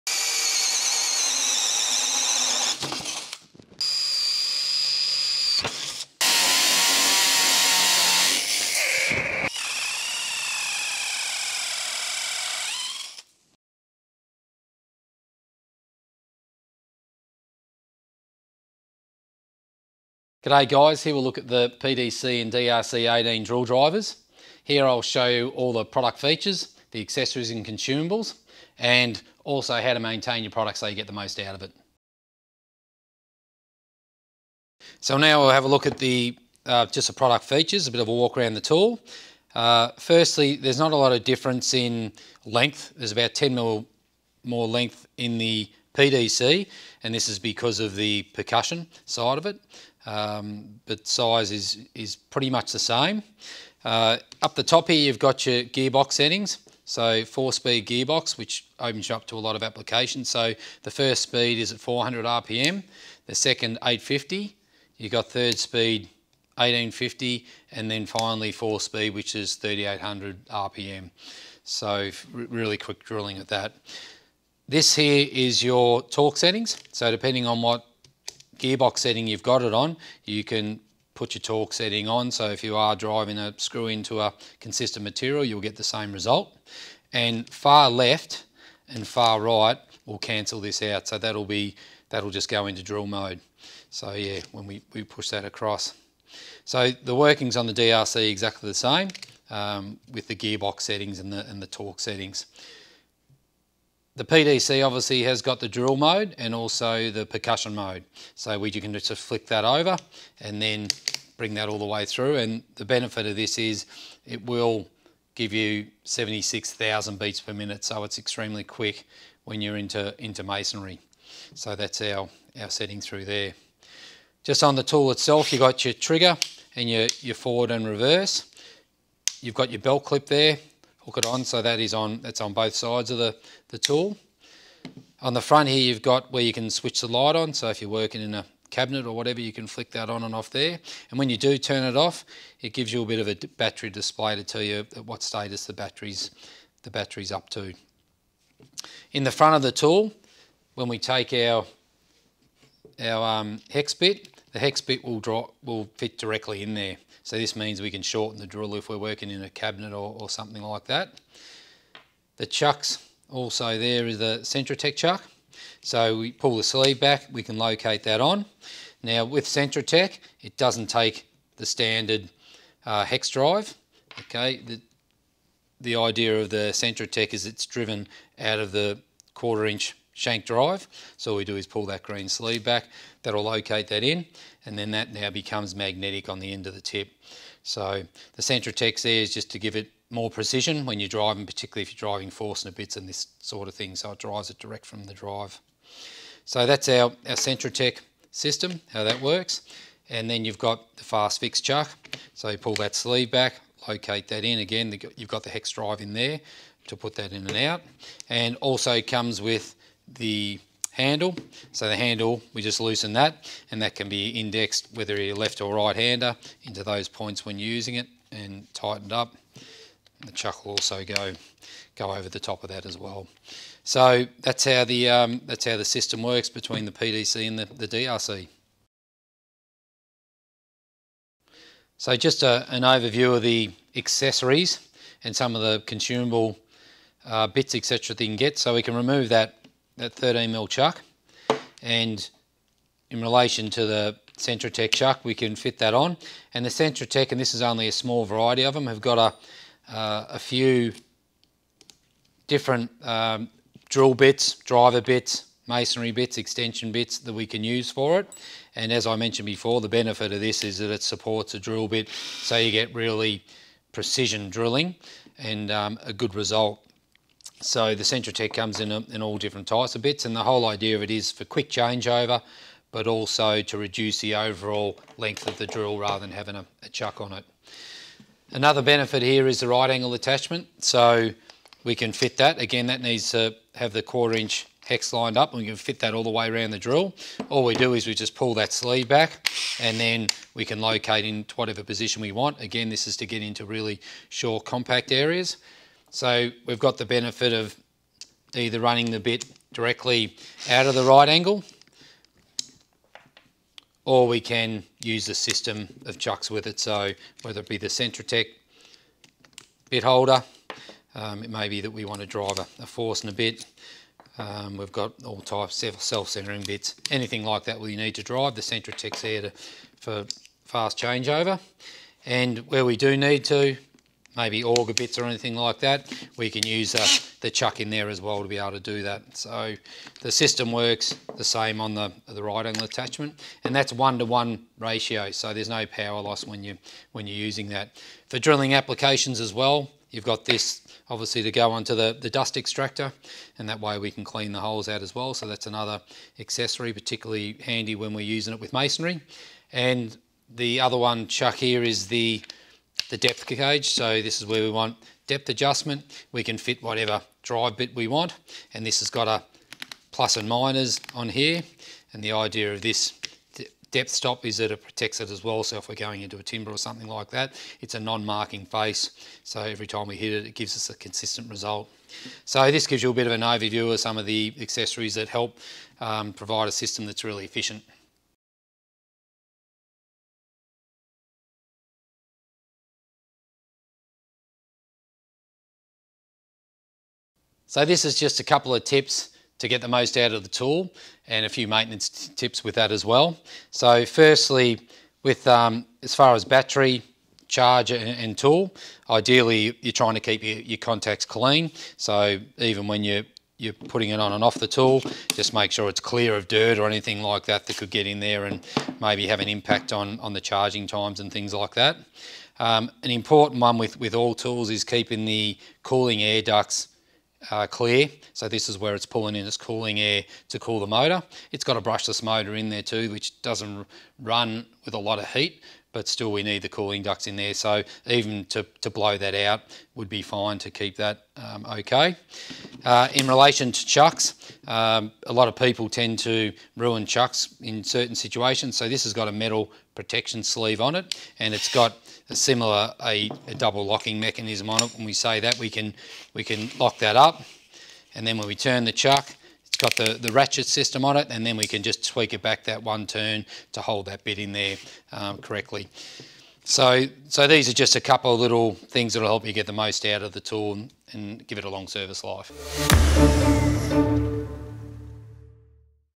Complete guide to the PDC & DRC 18 Drills
The EC-TEC brushless motor puts these drills in a class of their own in terms of torque and speed. The brushless motor is very energy efficient, delivering longer run times and fewer trips to the charger. 3,800 rpm at full speed means the drills are never short of power to get the toughest holes drilled. Combined with the FastFix chuck system, these drills can handle various jobs with ease. Simply click off the chuck and add a right angle attachment, socket adapter or depth stop chuck to power through any job.